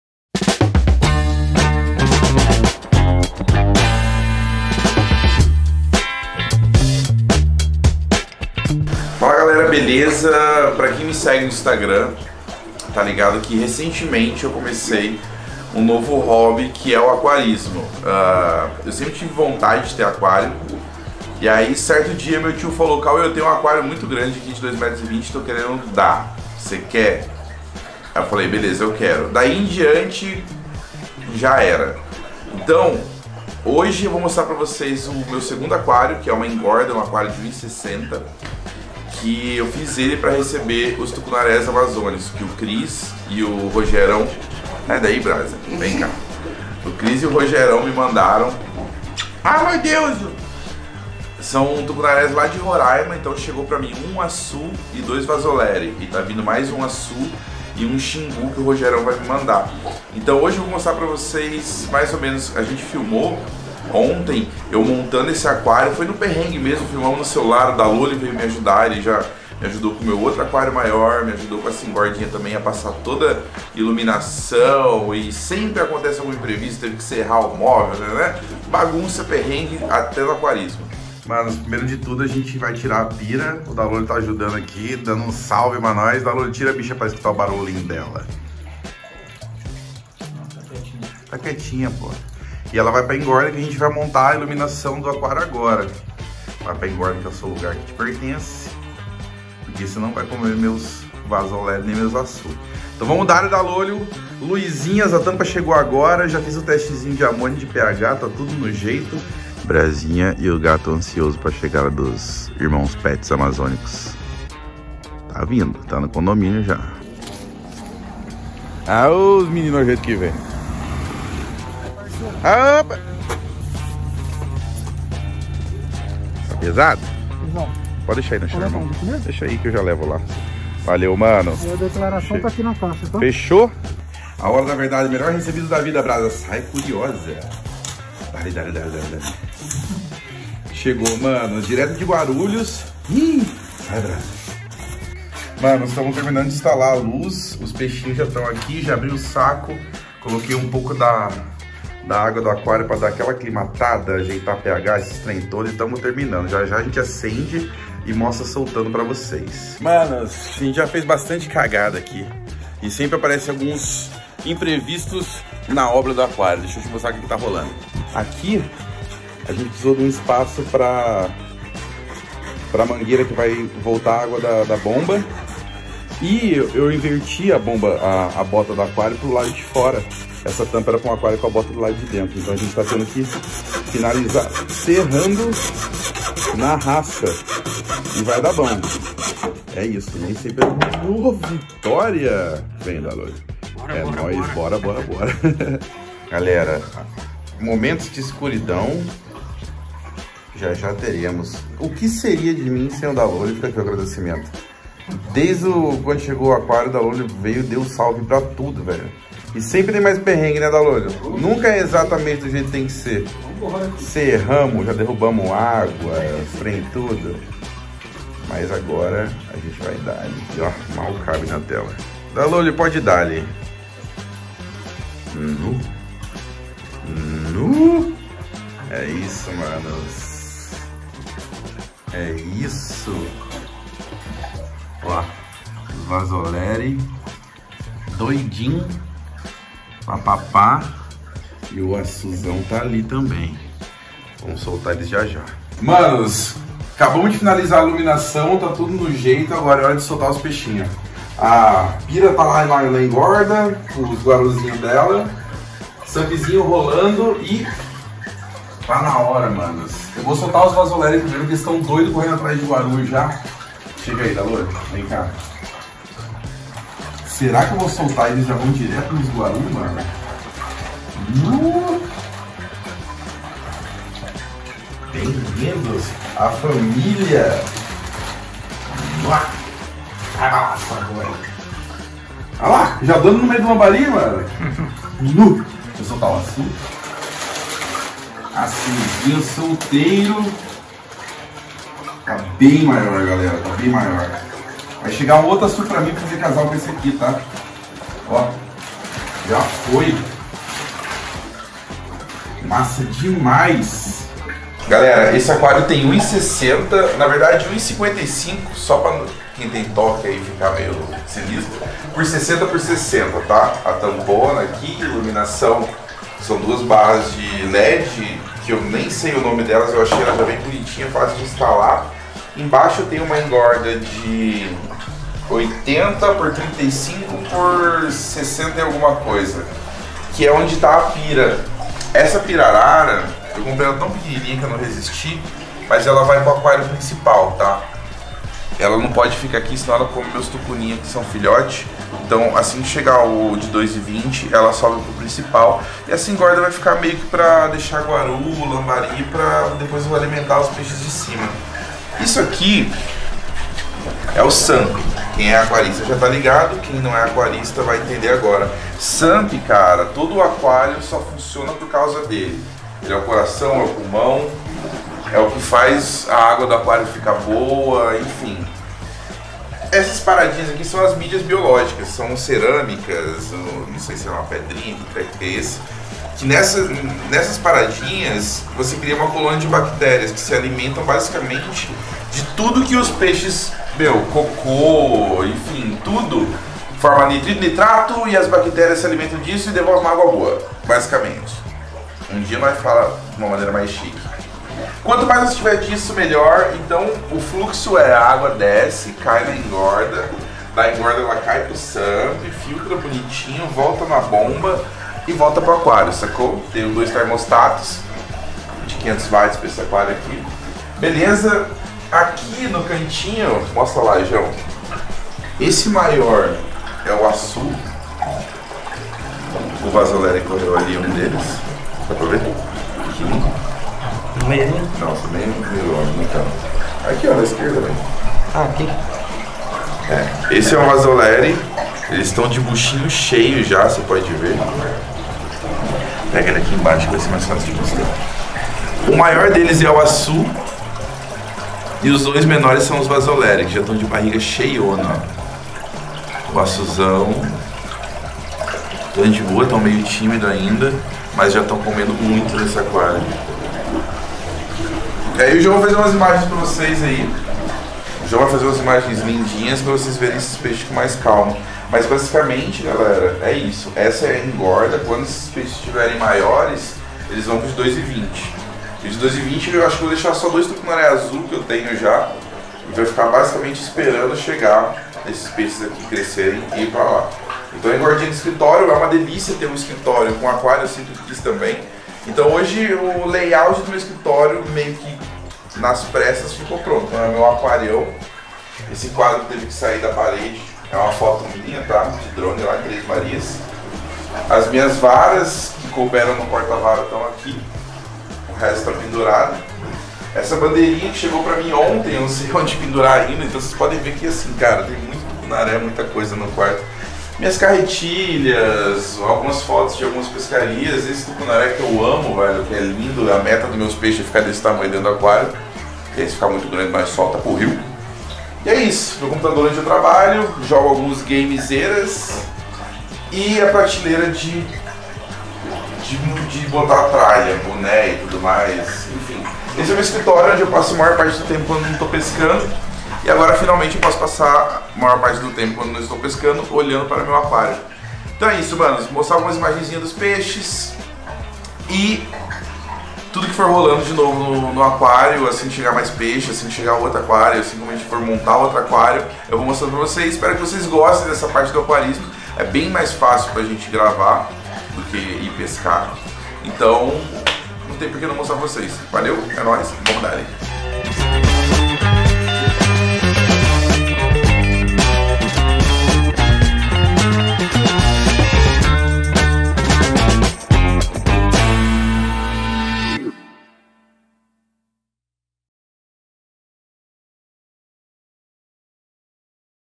Como é meu aquário de engorda! - SlowFishing
Para quem não tá ligado, agora eu também tenho aquários em casa.

Hoje vocês vão conhecer o menor deles, que fica no meu escritório, uma engordinha de 1,60 metros feita especialmente para receber os peixes menores antes de irem para o aquário principal!

Índice do vídeo:
0:00 Inscreva-se no canal!
0:05 Como é meu aquário de engorda! - SlowFishing
9:20 Valeuu!